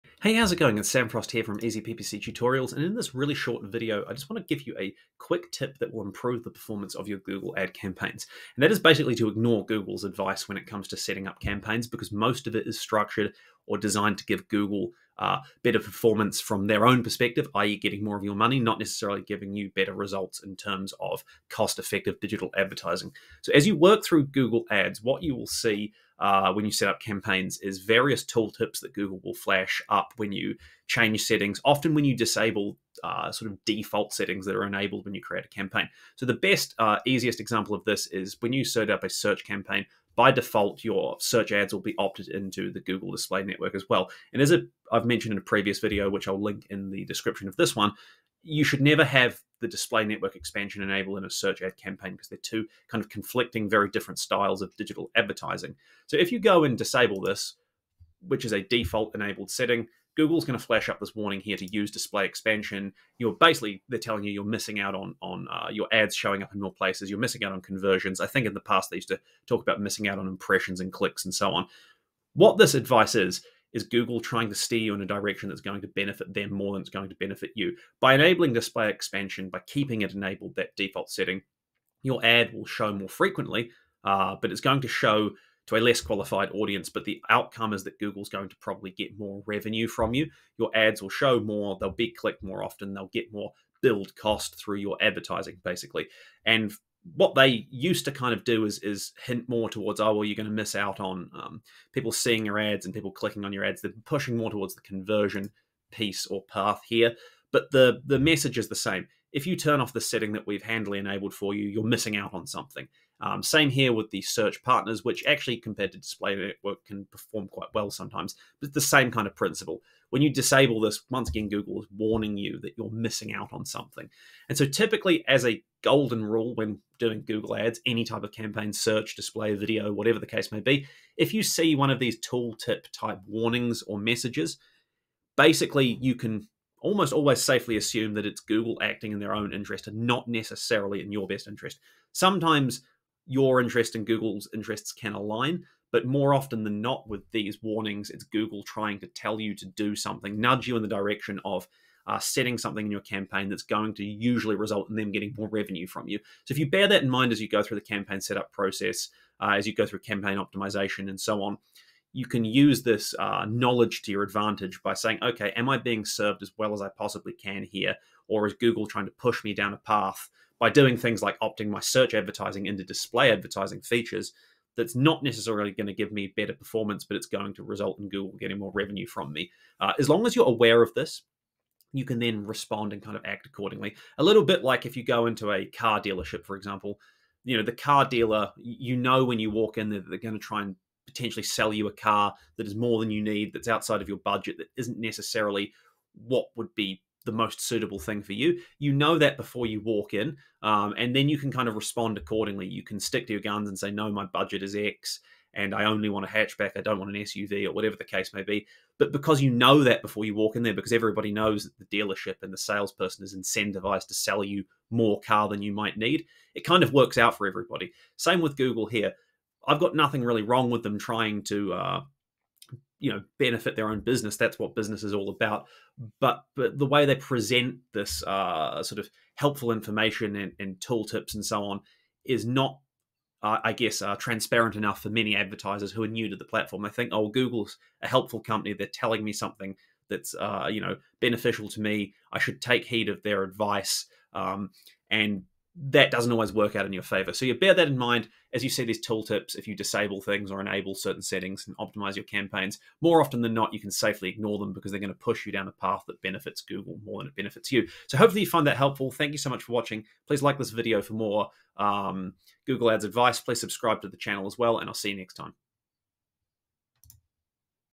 How To Improve Google Ads Performance By Ignoring Google's Advice
Here's a quick "hack" to improve the performance of your Google Ads - ignore most of Google's helpful advice.

The majority of the advice that Google gives you within Google Ads is designed to work to "Big G's" advantage and not yours. Sometimes the advice is mutually beneficial, but more often than not it isn't.

In this video I give examples of the kinds of in-platform advice and hints that Google gives, and why it should be ignored and discarded in most instances if you want the best outcome for YOUR business (not Google's).